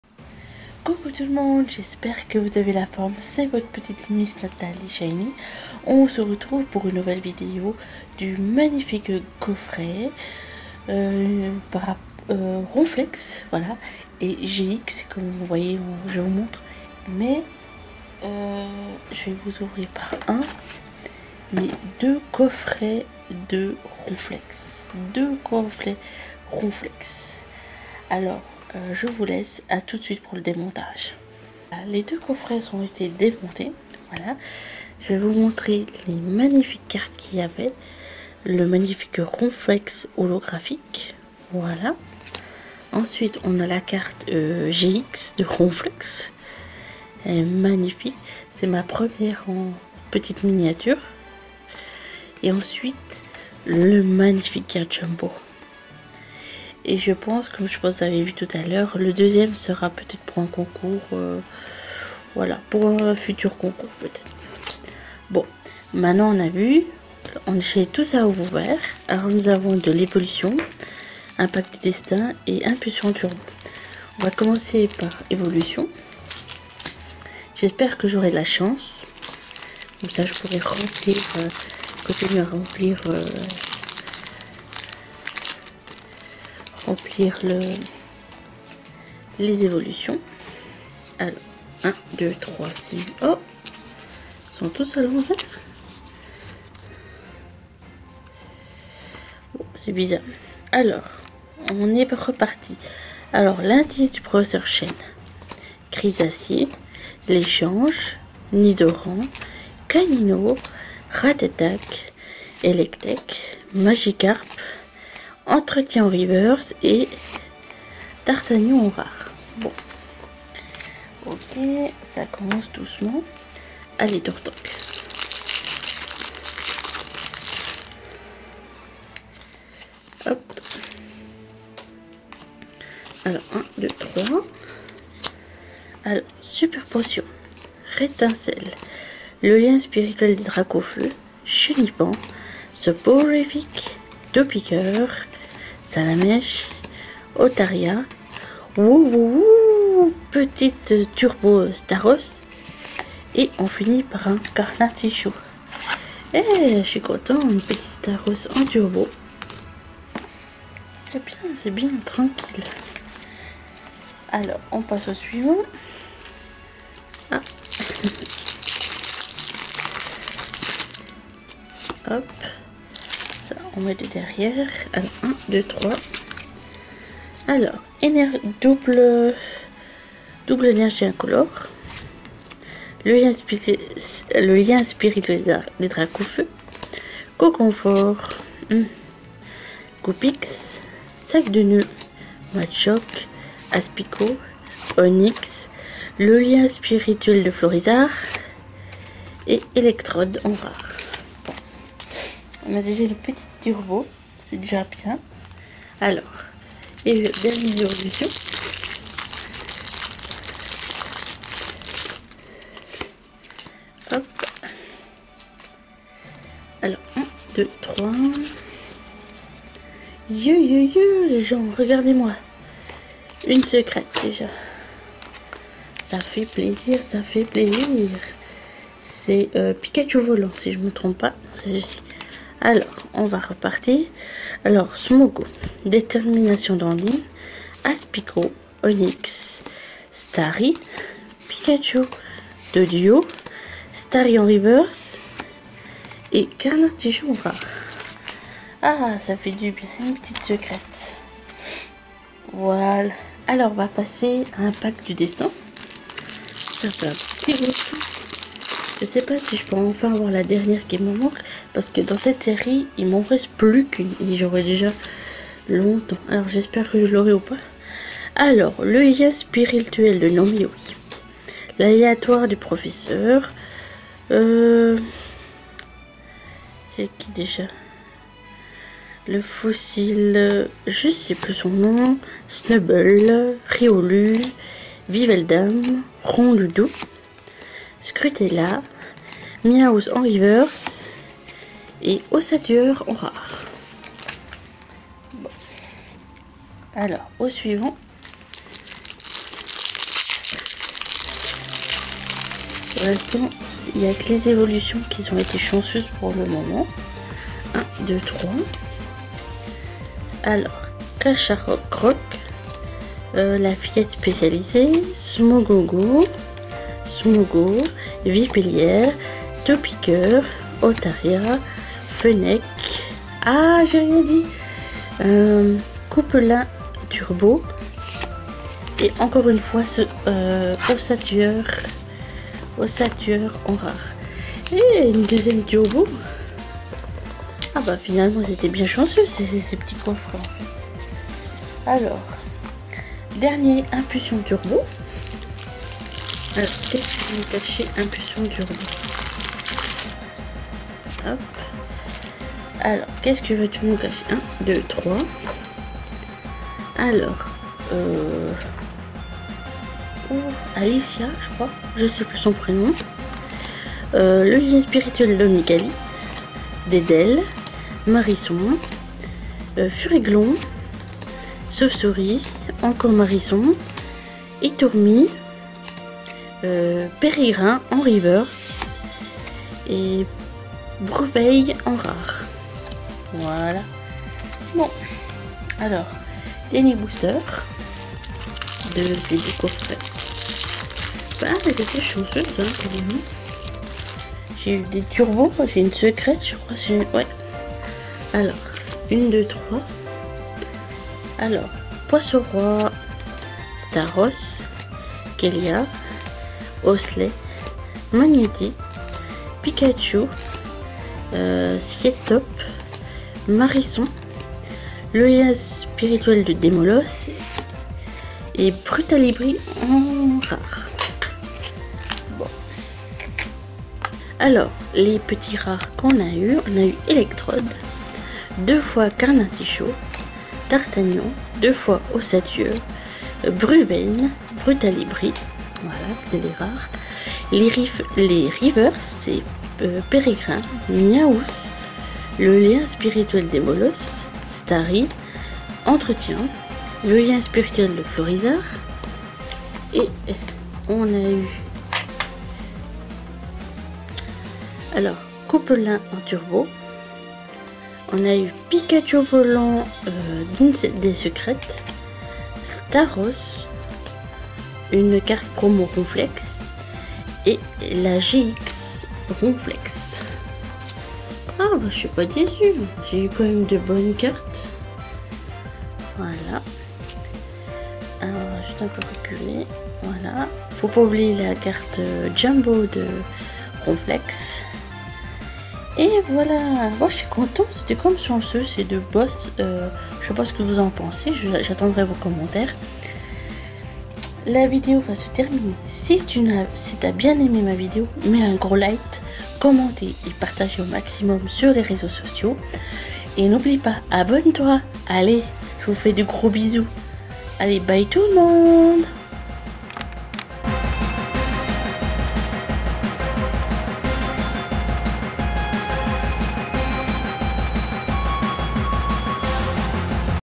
OUVERTURE DU COFFRET RONFLEX GX
Salut tous les amis nouvelle vidéo ouverture deux coffrets de ronflex GX et j’ai eu de jolie utras